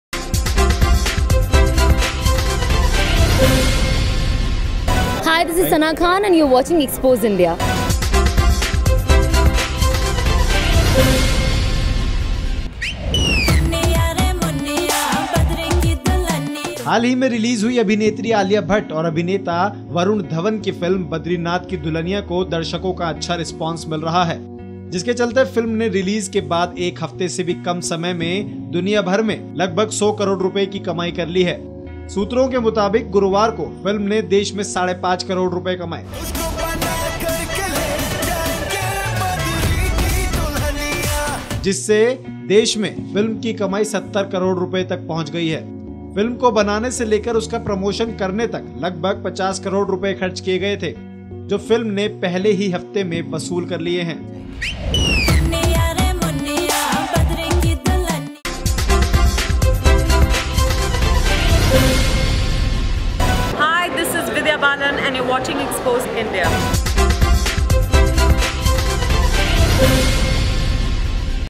100 करोड़ की हुई 'बद्रीनाथ की दुल्हनिया'
अभिनेत्री आलिया भट्ट और अभिनेता वरुण धवन की फिल्म 'बद्रीनाथ की दुल्हनिया' को दर्शकों का अच्छा रिस्पॉन्स मिल रहा है जिसके चलते फिल्म ने रिलीज के बाद एक हफ्ते से भी कम समय में दुनियाभर में  लगभग 100 करोड़ रुपये की कमाई कर ली है